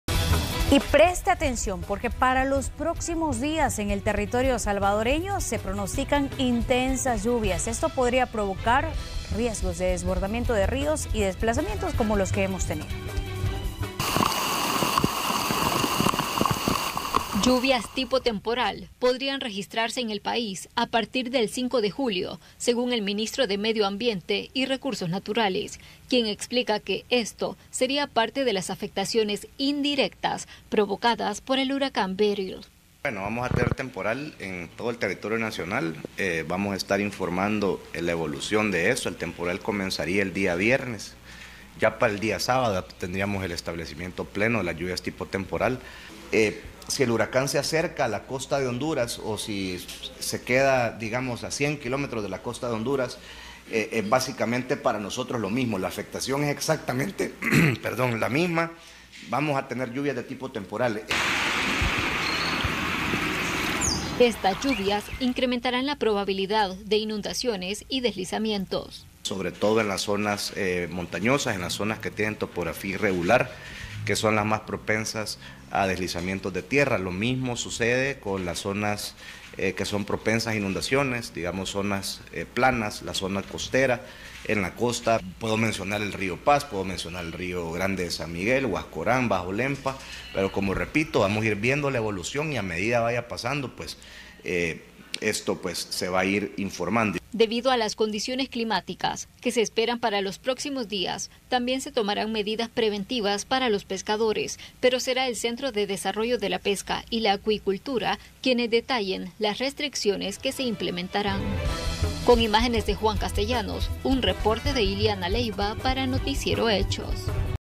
El Salvador podría registrar lluvias tipo temporal
Este fenómeno podría provocar nuevas inundaciones y deslizamientos en zonas vulnerables.